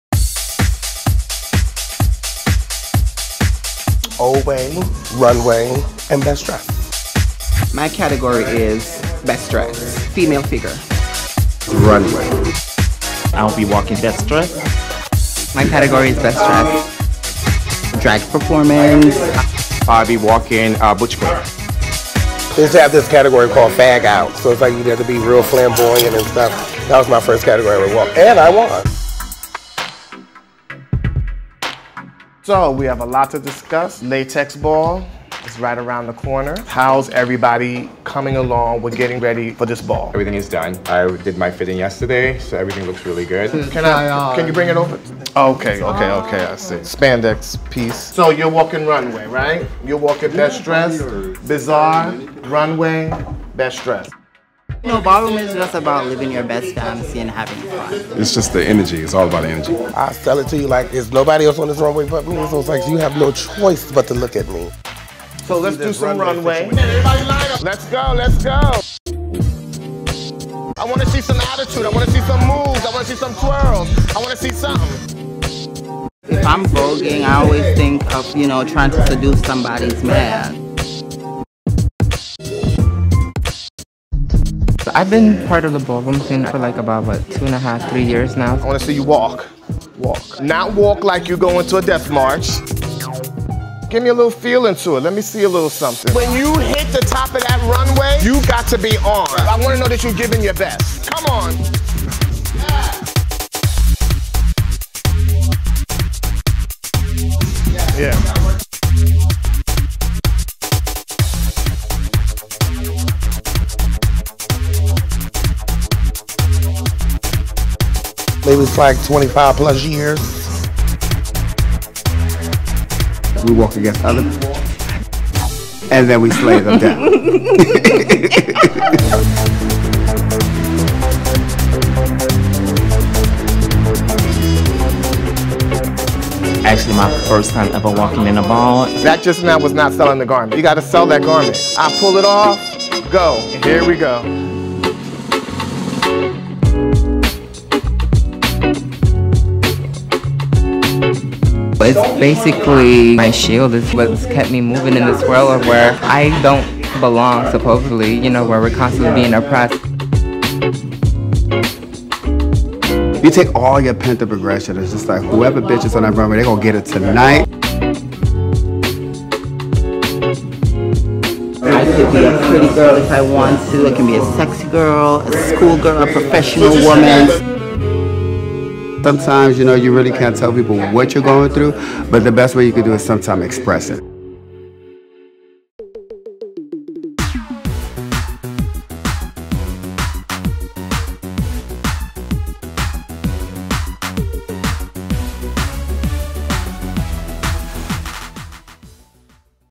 Bringing On The Moves | Latex Ball | Logo TV
As the LatexBall approaches, houses gather to prepare to bring it on the runway. We got to hang with the House of Soulja for their rehearsal at GMHC for the big night! It’s time to serve and get your 10s, the house mother and father want their children to be READY.

Stay up-to-date with fierce content before and after the ball on Logo’s social media and LogoTV.com!

The Latex Ball is the largest gathering in the world for the House and Ball community, and attendees include hundreds of the community’s members who participate in the festivities; fashion models, designers, renowned and photographers; and leadership from community-based organizations.

More than 2,000 people from around the world are expected to attend the 29th annual Latex Ball, produced by Gay Men’s Health Crisis (GMHC). Taking place on Saturday, June 22, at Terminal 5 in New York City.